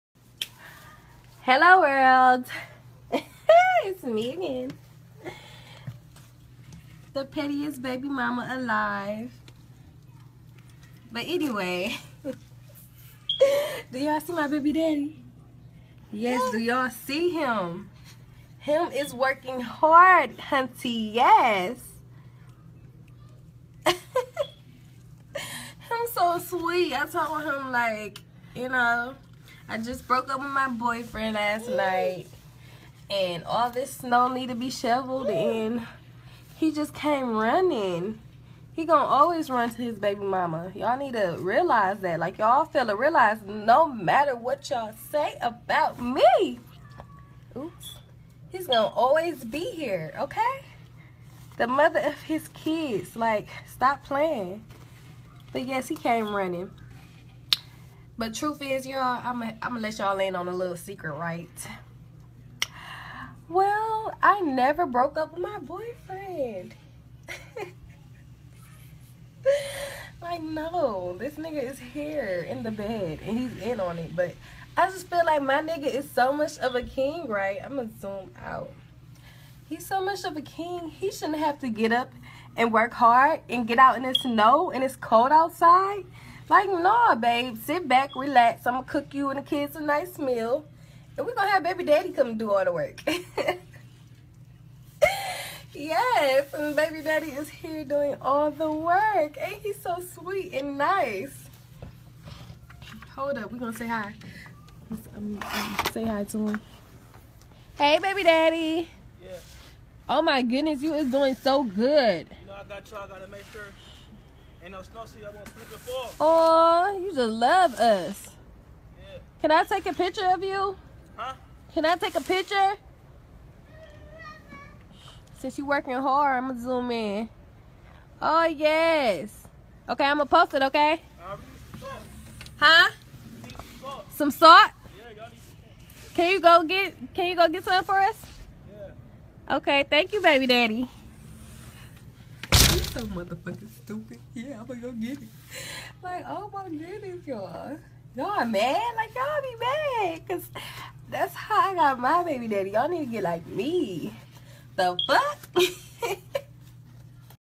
WTF ‼️ PETTY BABY MAMA CLOWNS BABY DADDY WHILE HE SHOVELS HER SNOW! (Skit)
WTF ‼️ PETTY BABY MAMA CLOWNS 🤡 BABY DADDY WHILE HE SHOVELS HER SNOW! 🌨❄️☃️😱